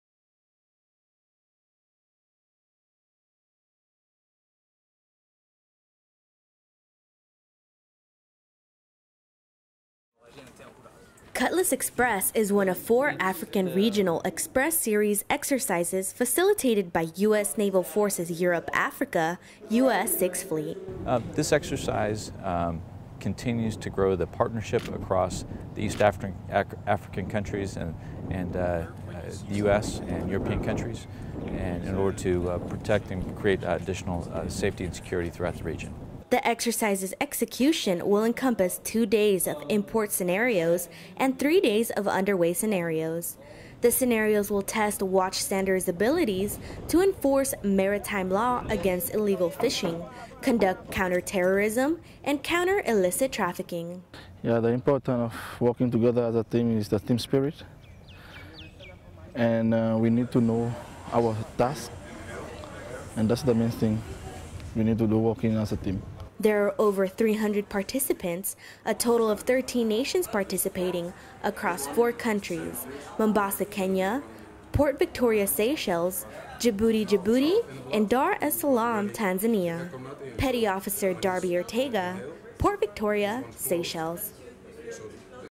Cutlass Express 2013 OpeningCeremony
Exercise Cutlass Express 2013 is a multinational maritime exercise in the waters off East Coast Africa to improve cooperation, tactical expertise and information sharing among East African maritime forces in order to increase maritime safety and security in the region.